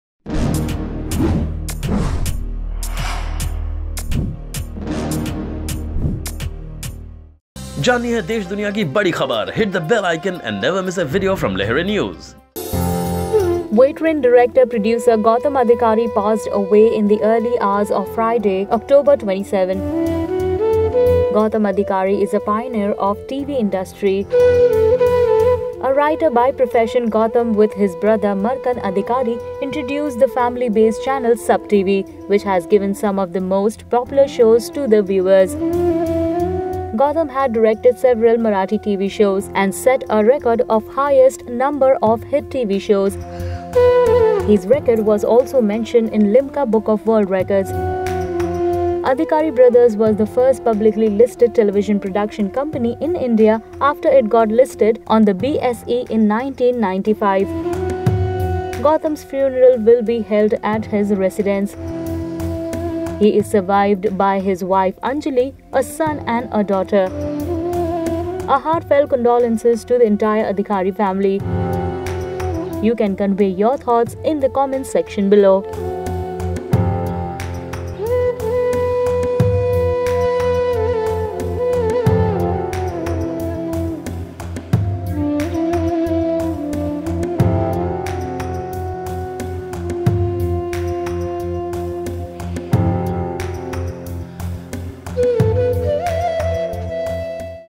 Gautam Adhikari, Co-Owner of Leading TV Channel SAB TV, Passes Away At 67 | Lehren News
Veteran director/producer Gautam Adhikari passed away in the early hours of Friday, October 27. To know more check out the video.